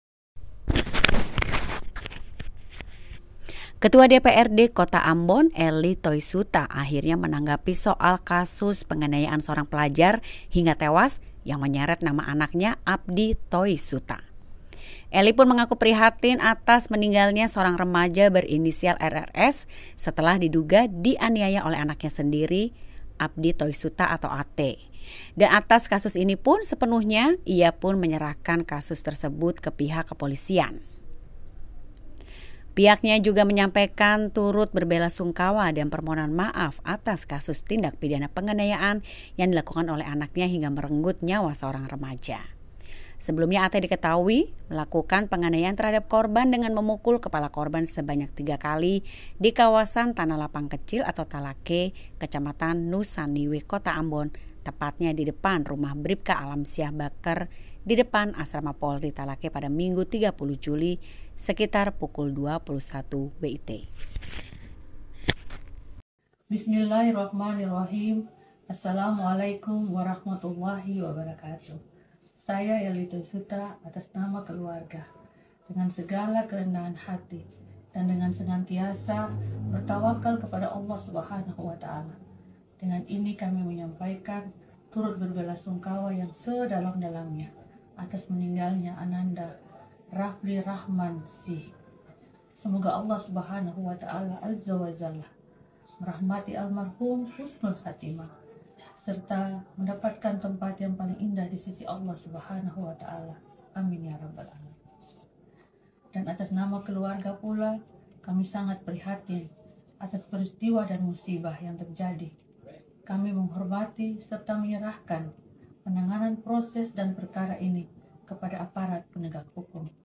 Anaknya Bikin Pemuda T3was, Ketua DPRD Kota Ambon Bersuara
Ketua DPRD Kota Ambon Elly Toisuta akhirnya menanggapi soal kasus penganiayaan seorang pelajar hingga tewas, yang menyeret nama anaknya, Abdi Toisuta (AT).

Elly mengaku prihatin atas meninggalnya seorang remaja berinisial RRS (16) setelah diduga dianiaya anaknya, AT (25).

Dan atas kasus ini ia pun sepenuhnya menyerahkan kasus tersebut ke pihak kepolisian.

"Kami turut prihatin atas apa yang terjadi, mewakili keluarga kami menghormati dan menyerahkan penanganan proses perkara ini kepada aparat penegak hukum," kata Elly.

Pihaknya pun menyampaikan turut berbelasungkawa dan permohonan maaf atas kasus tindak pidana penganiayaan yang dilakukan oleh anaknya hingga merenggut nyawa seorang remaja.

"Atas nama keluarga saya sampaikan permohonan maaf sedalam-dalamnya, dan turut berbelasungkawa atas meninggalnya korban," ucapnya.

Sebelumnya AT diketahui melakukan penganiayaan terhadap korban dengan memukul kepala korban sebanyak tiga kali di kawasan Tanah Lapang Kecil (Talake), Kecamatan Nusaniwe (Kota Ambon), tepatnya di depan rumah Bripka Alamsyah Bakker depan Asrama Polri Talake, Minggu (30/7/2023) sekitar pukul 21.00 WIT.

Laporan: Joanita Ary
Editor Video: Rangga Baskoro
Sumber: YT Tribun Ambon/IG net2netnews